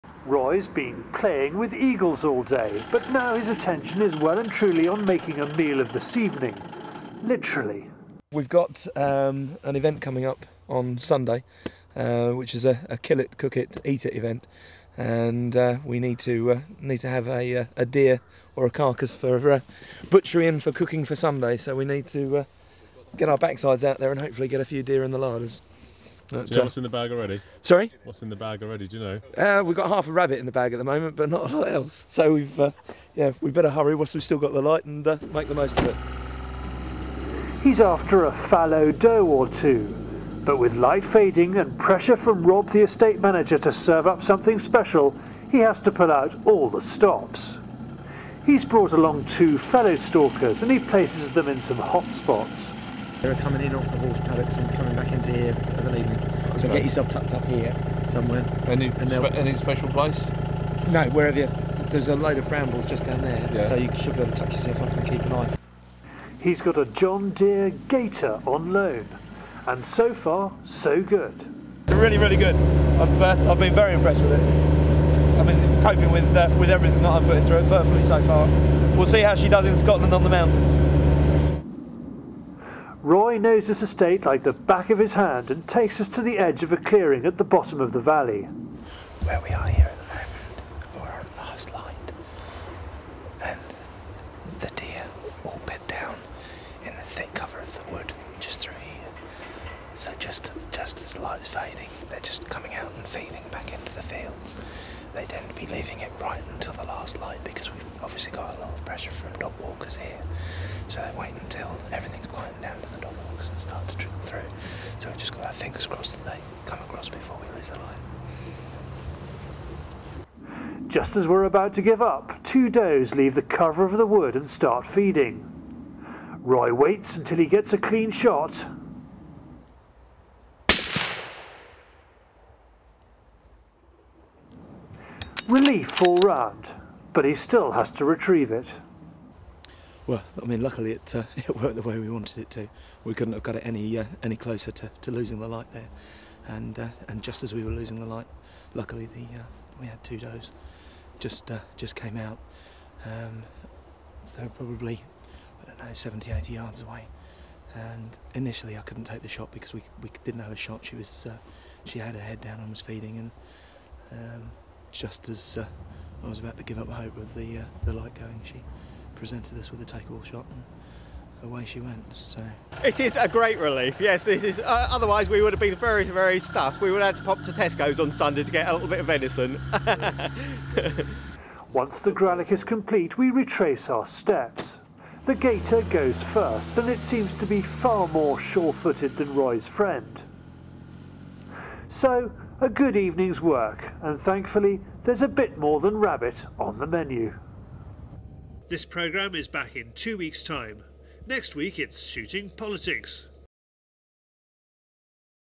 Stalking fallow does in Sussex
Now when you're trying to impress dinner guests it can be nice to serve up a bit of game. At Buxted Park Hotel they're hosting a kill-it-cook-it-eat-it evening. The trouble is at the moment the 15 diners are going to have to fight over a small rabbit. They call in Roy Lupton, late in the day, to sort them out with a bit of venison. This film first appeared in Fieldsports Britain, episode 15.

▶ Shares
To find out how to buy shares in Fieldsports Channel,